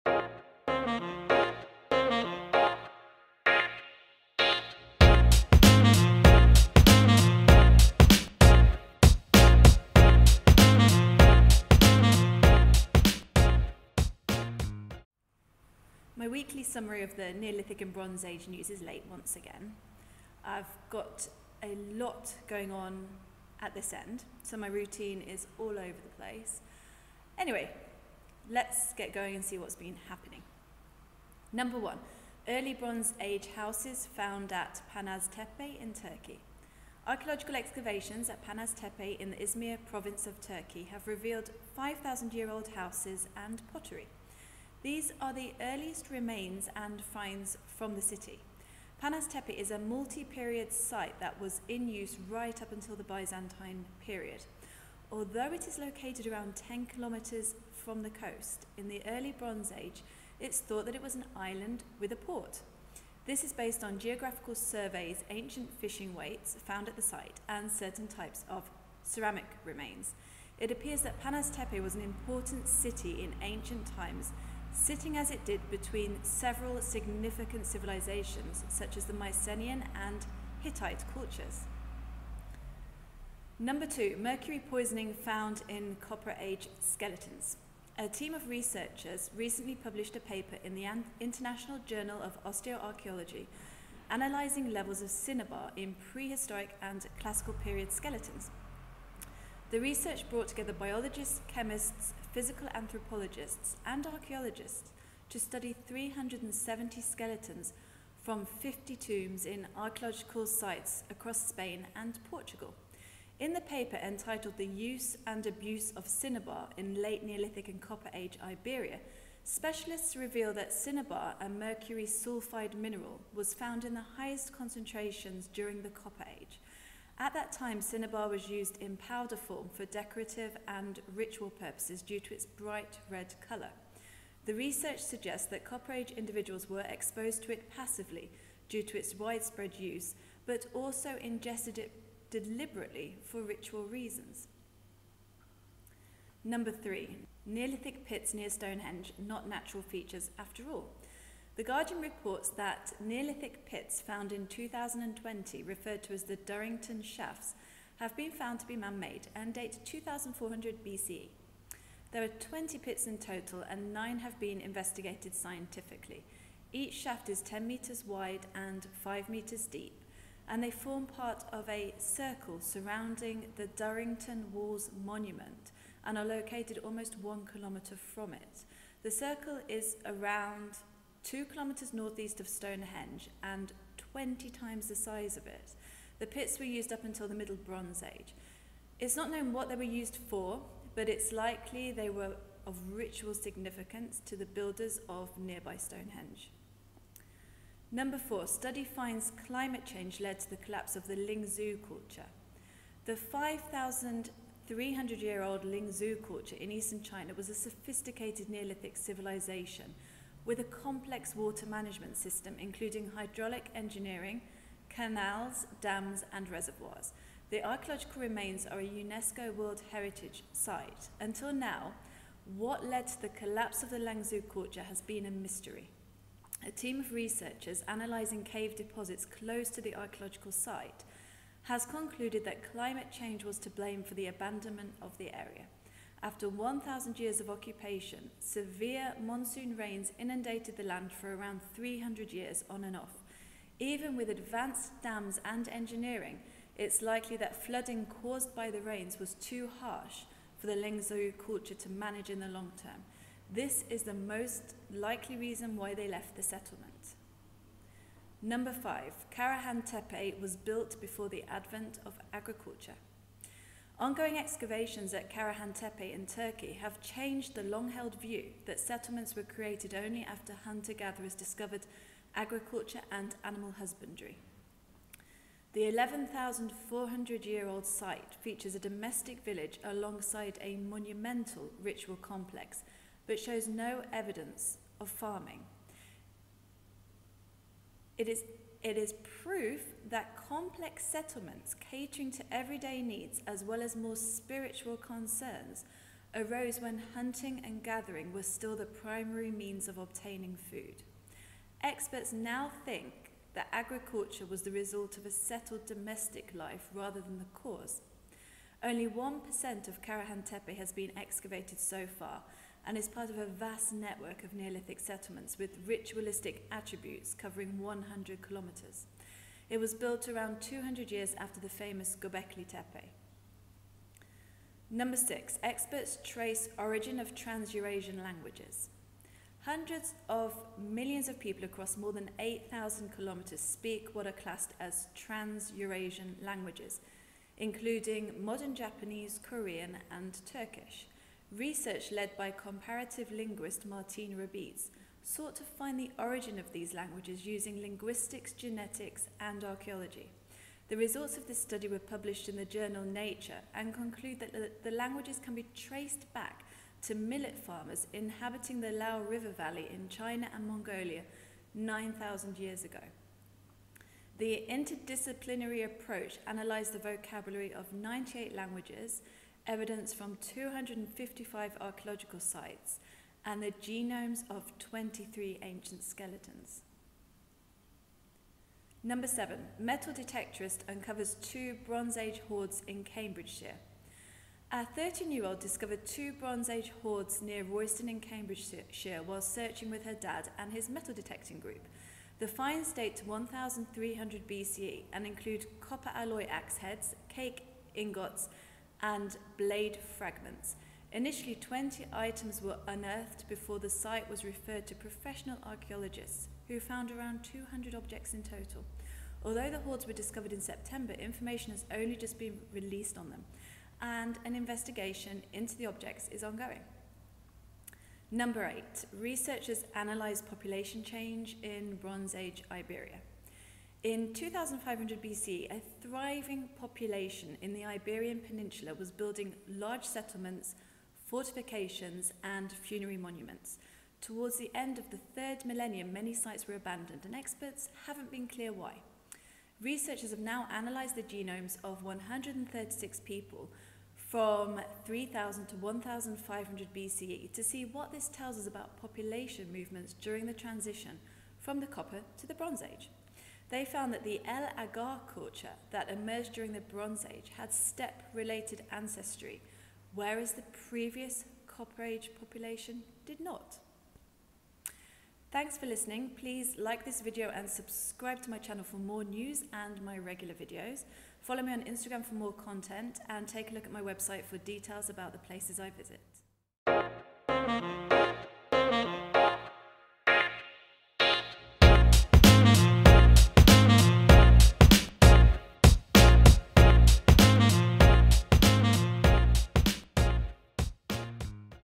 Neolithic and Bronze Age News | 29th November 2021
In this video I bring you eight news stories from the past two weeks including those mysterious artificial pits near Stonehenge that everyone is talking about!

00:00 Introduction
00:32 Early Bronze Age houses found at Panaztepe in Turkey
01:23 Mercury poisoning found in Copper Age skeletons
02:26 Neolithic pits near Stonehenge not natural features after all
03:17 Study finds climate change led to the collapse of the Liangzhu culture
04:18 Karahan Tepe was built before the advent of agriculture
05:24 Experts trace origin of Transeurasian languages
06:17 Metal detectorist uncovers two Bronze Age hoards in Cambridgeshire
06:57 Researchers analyse population change in Bronze Age Iberia

✨  SOURCES

Early Bronze Age houses found at Panaztepe in Turkey.

Neolithic pits near Stonehenge not natural features after all

Study finds climate change led to the collapse of the Liangzhu culture

Karahan Tepe was built before the advent of agriculture

Metal detectorist uncovers two Bronze Age hoards in Cambridgeshire

✨  MUSIC CREDIT

✨  PHOTOGRAPH CREDITS

Durrington Walls, credit: Ethan Doyle White

Liangzhu ancient city site, credit: Siyuwj
Fertile Crescent, credit: Fraxin
Transeurasian lanuages, credit: Satoshi Kondo
Al Argar culture pottery vessel, credit: Miguel Hermoso Cuesta

Panazatepe, credit: Erkmen Senan

Castro do Zambujal, Copper Age site in Portugal, credit: Vitor Oliveria
Royston church, credit: John Salmon